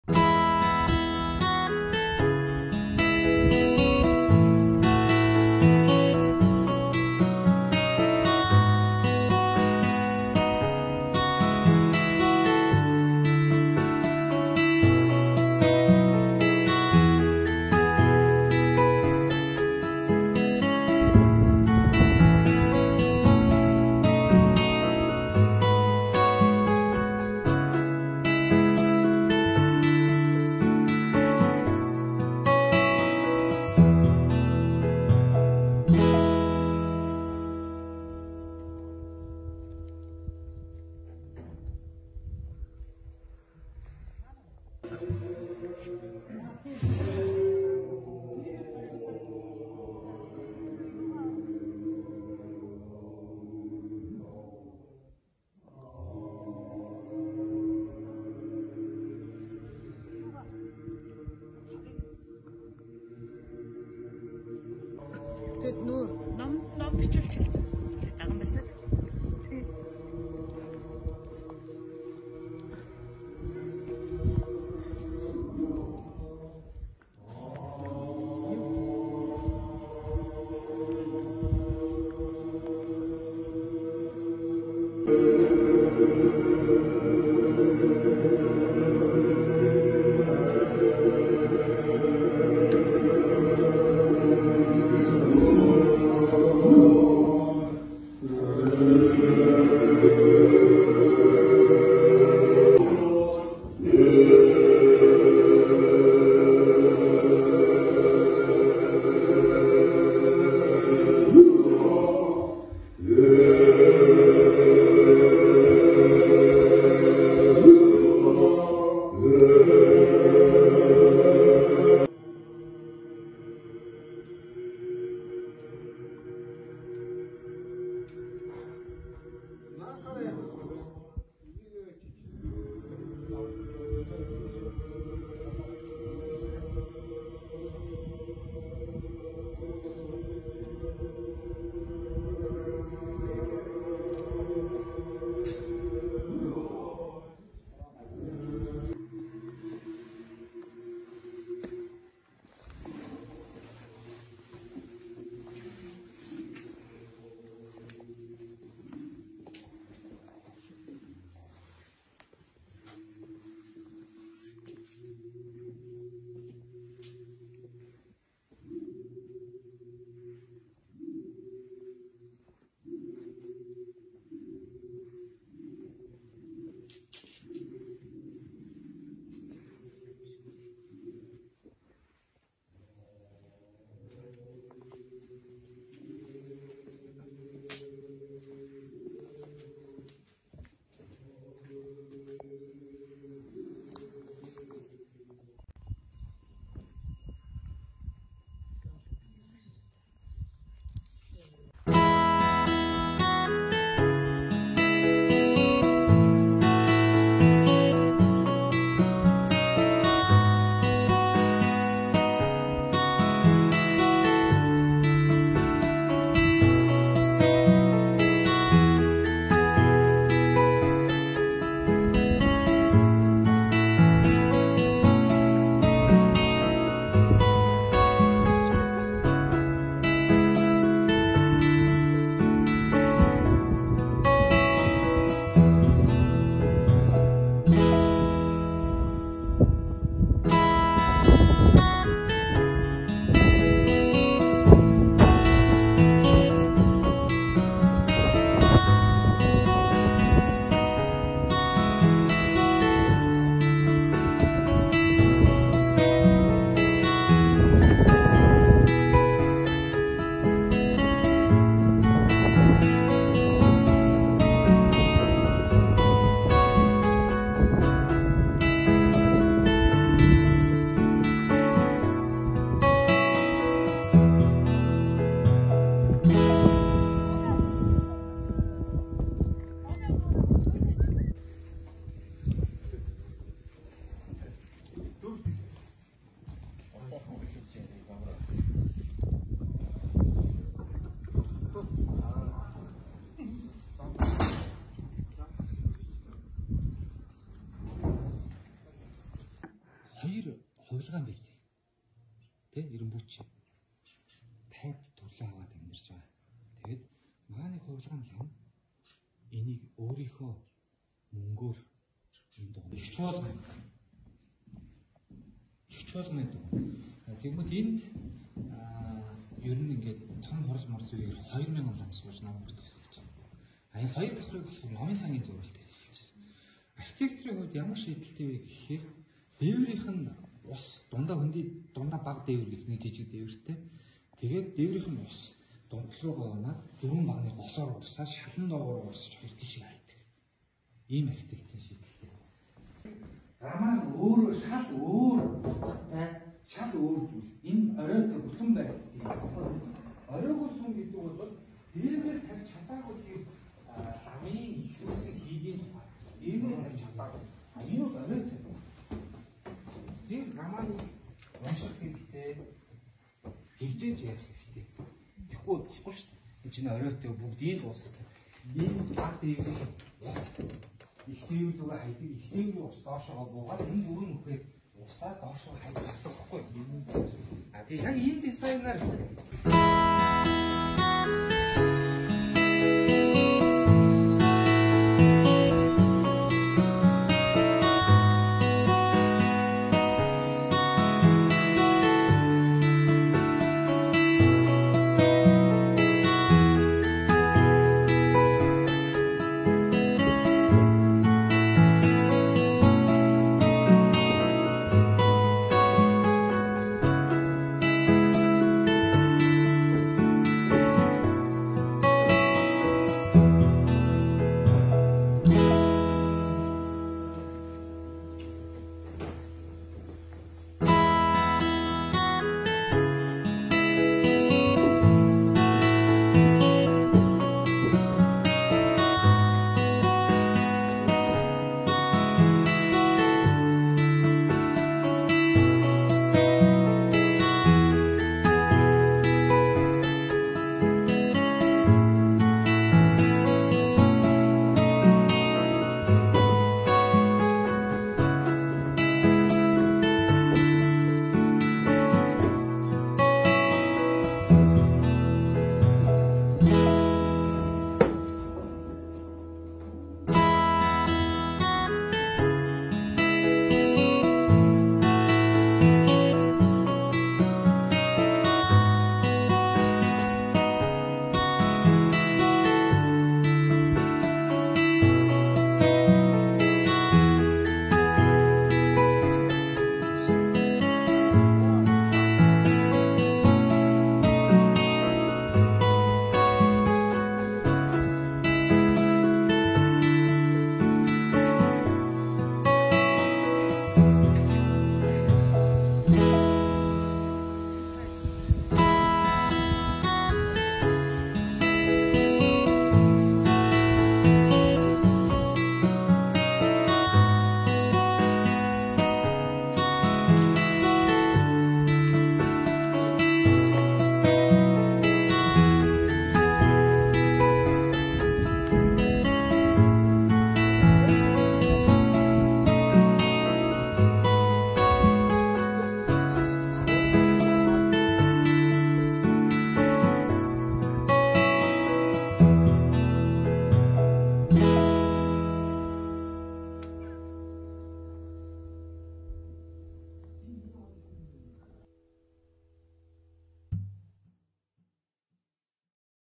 Амарбаясгалант хийд
Сэлэнгэ аймагт орших Амарбаясгалант хийдэд цамын хурлын үеэр зочилж очсон бичлэгээ хүргэж байна.